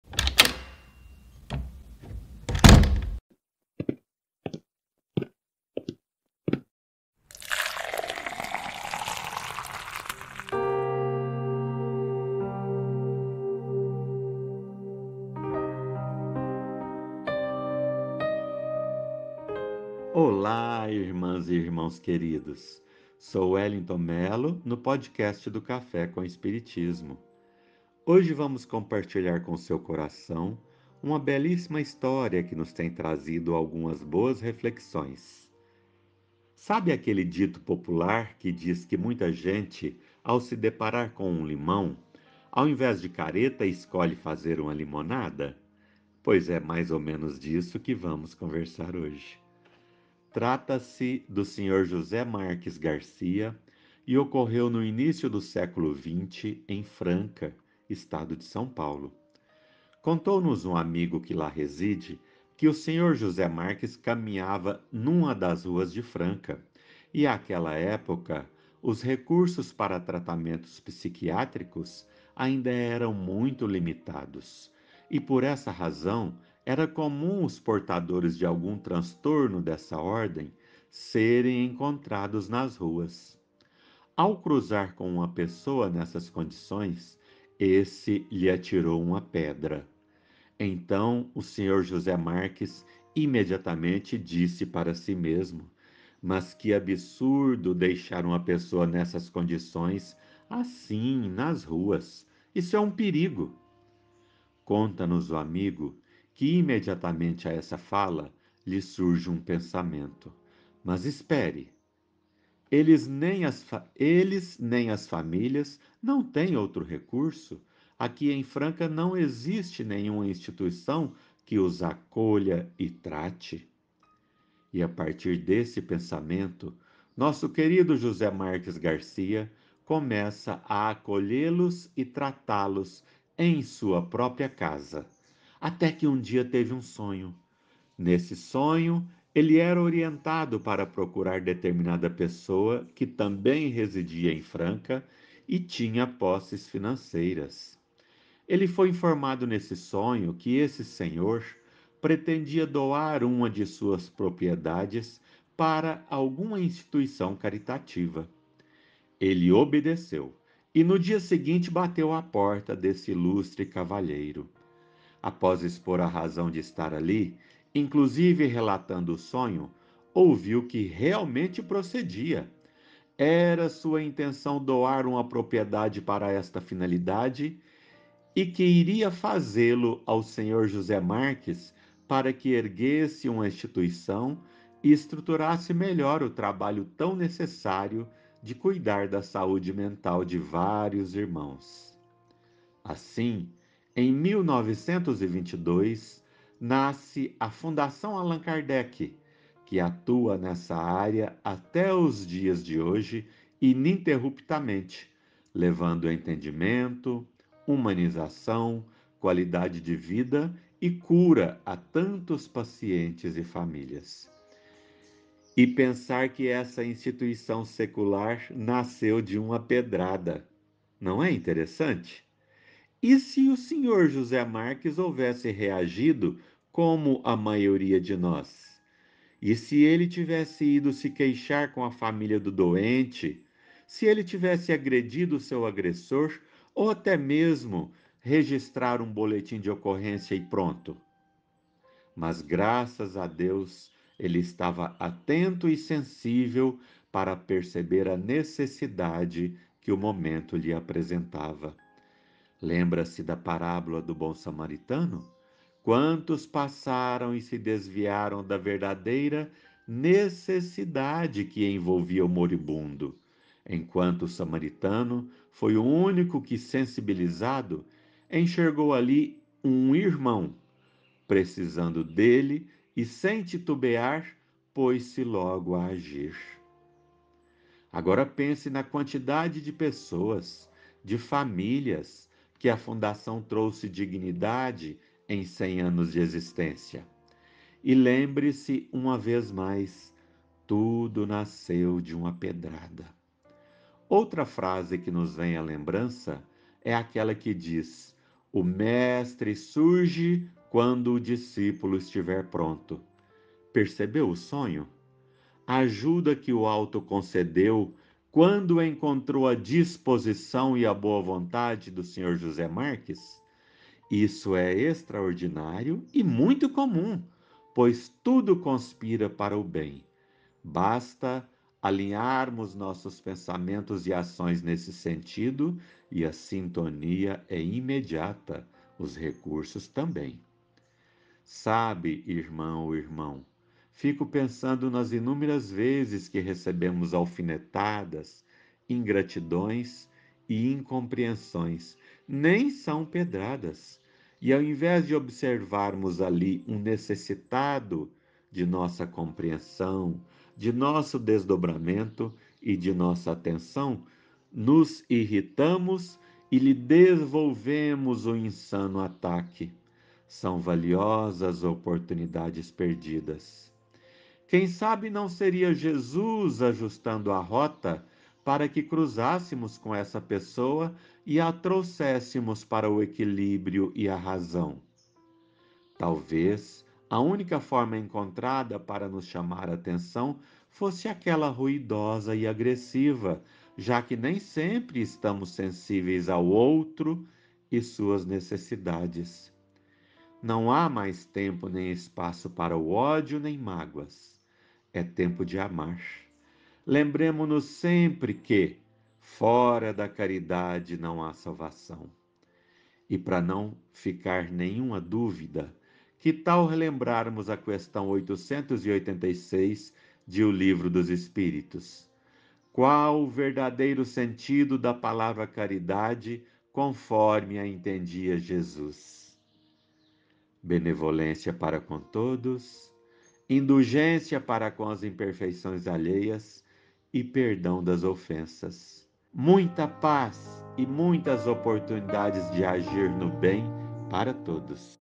CaféComEspiritismo: 1529 - Nunca devolver o mal - Wellington Melo
Olá, pessoal!!

Mensagem: Nunca devolver o mal
Questão: 886
Livro: O Livro dos Espíritos
Autor: Allan Kardec
Comentários: Wellington Melo